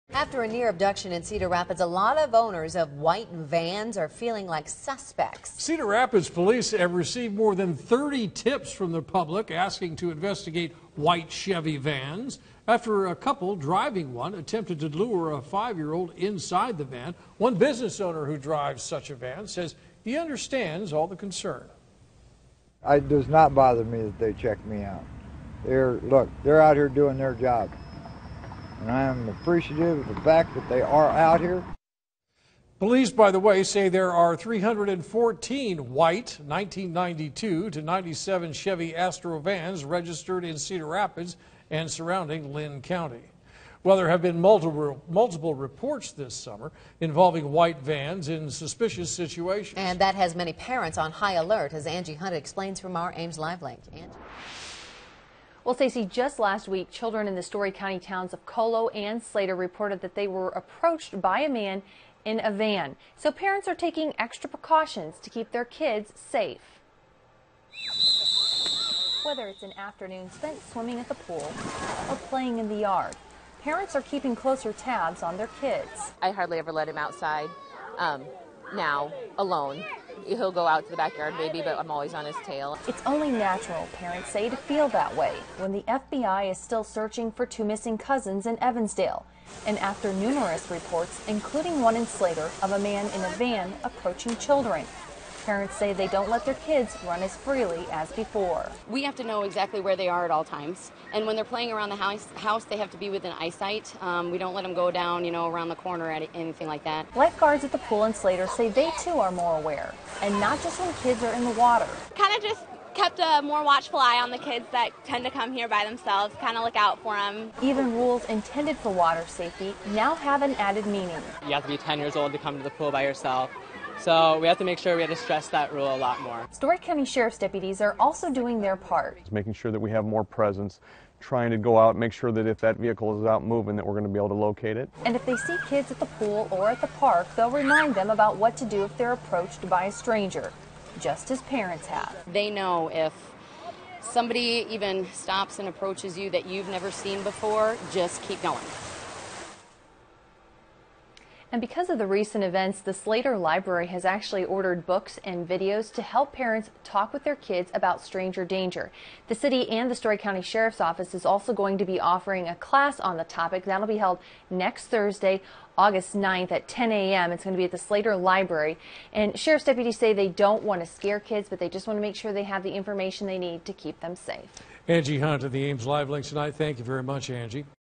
Suspicious van worries parents
Parents were on high alert Thursday after multiple reports about a suspicious white van over the summer.